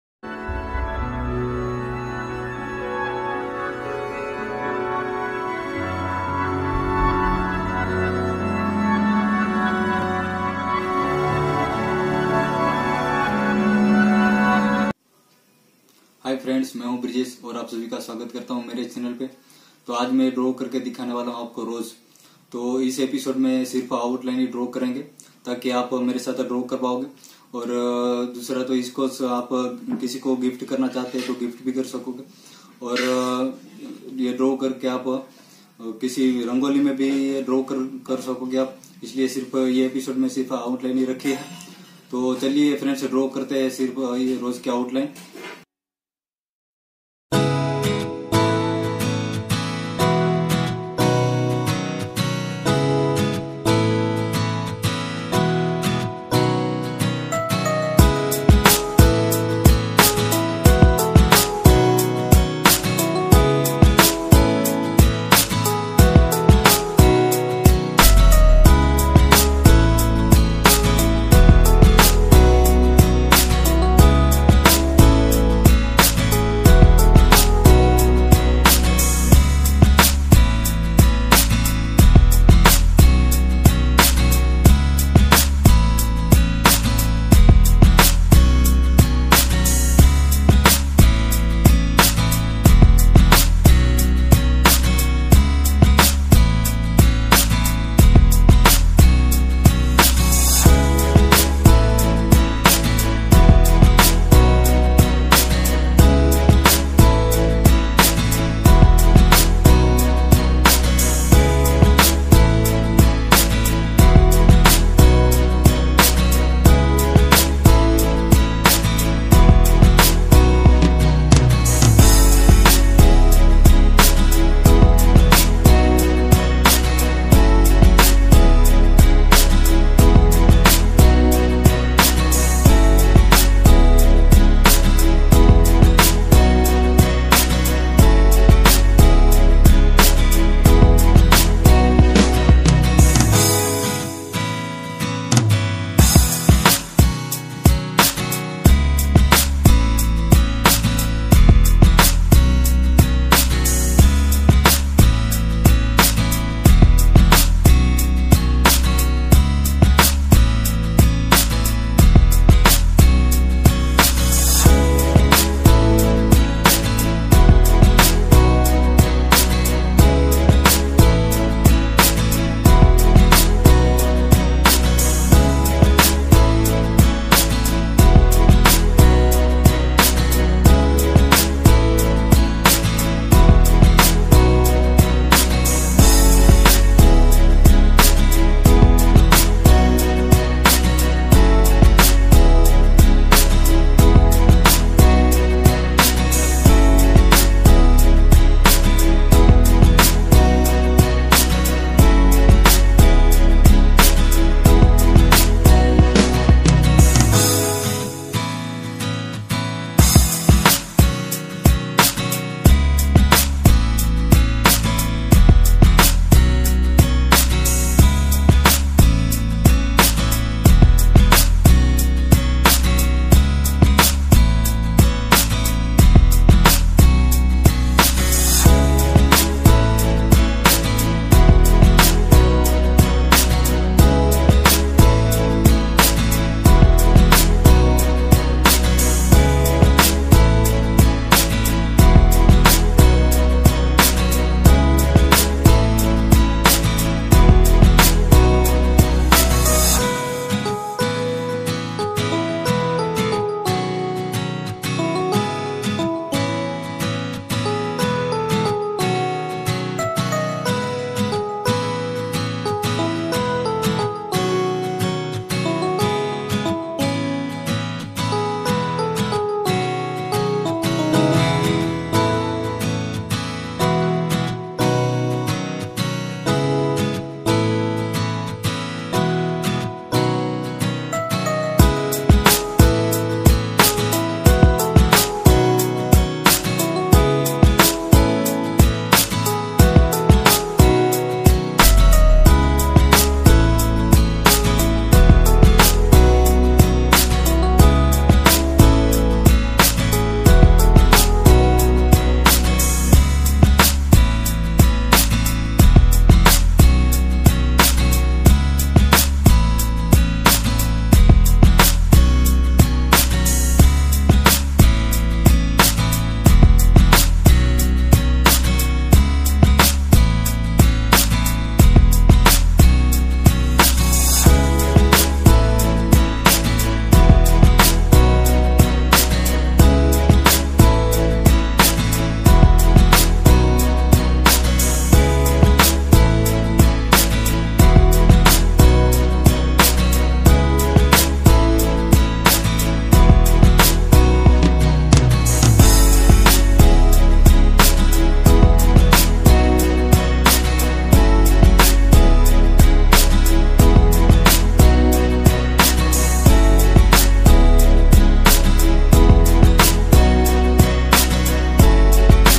Happy Rose Day painting Episode-1
Brijesh Art

About Video

Hii friends aap sabhi ka Swagat karta hun me

To Aaj me Aapko draw karke dikhane vala hai
Happy Rose day painting
Episode-1 me shirf outline hi draw karenge friends taki ye outline aap sabhi Rangoli me and orbhi jagah use ho sake